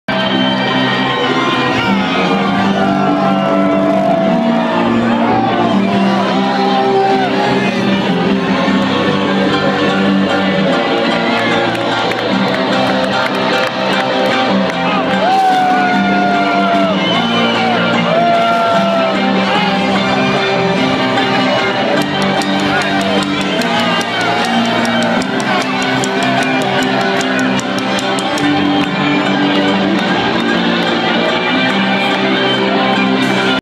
Mat "The Kid" Groll Crowd Surfing
Matt "The Kid" Groll crowd surfing at Houston's House of Blues during Michael Berry's tribute to Lynyrd Skynyrd featuring Captain Legendary Band on January 15, 2013.